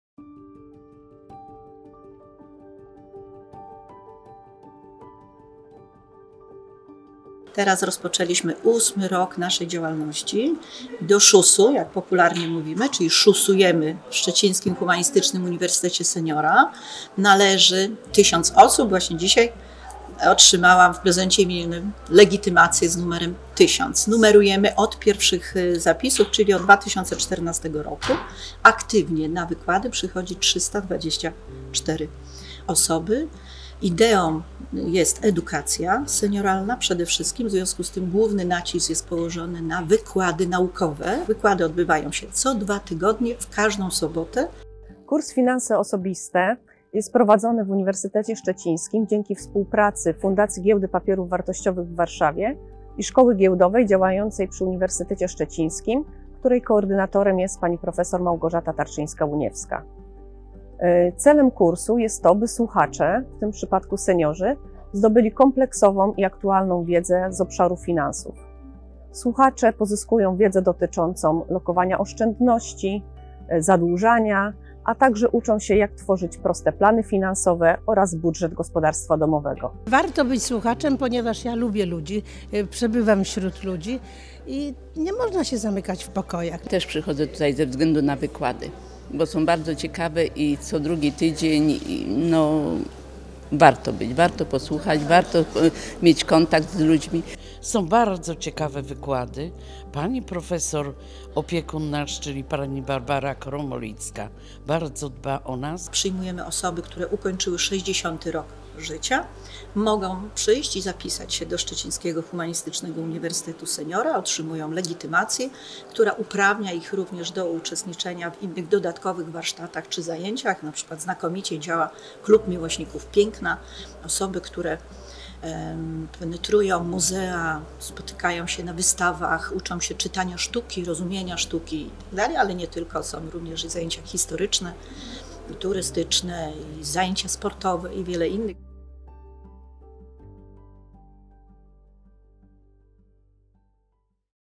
Szczeciński Humanistyczny Uniwersytet Seniora ma już 1000 słuchaczy
Coraz więcej studentów na Szczecińskim Humanistycznym Uniwersytecie Seniora!
Obejrzyj krótki film i dowiedz się więcej o możliwościach jakie daje Uczelnia.

Produkcja: UNIWIZJA – telewizja Uniwersytetu Szczecińskiego.